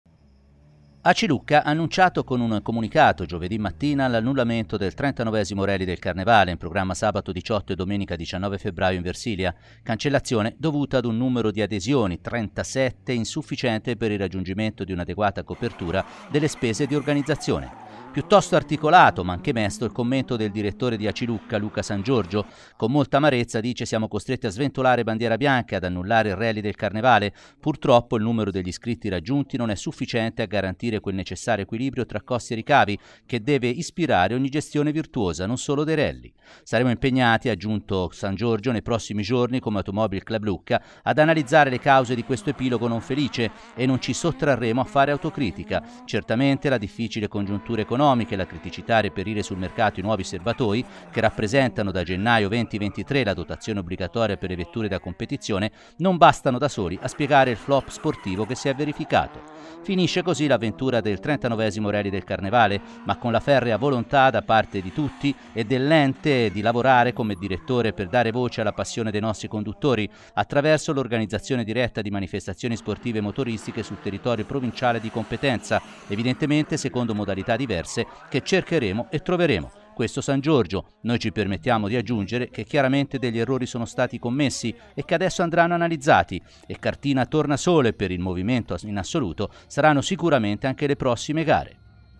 Solo 37 iscritti:, annullato il 39esimo Rally del Carnevale
ACI Lucca ha annunciato l’annullamento del 39° Rally del Carnevale, in programma sabato 18 e domenica 19 febbraio in Versilia. La cancellazione è dovuta ad un numero di adesioni (37) insufficiente per il raggiungimento di un’adeguata copertura delle spese di organizzazione.

NoiTv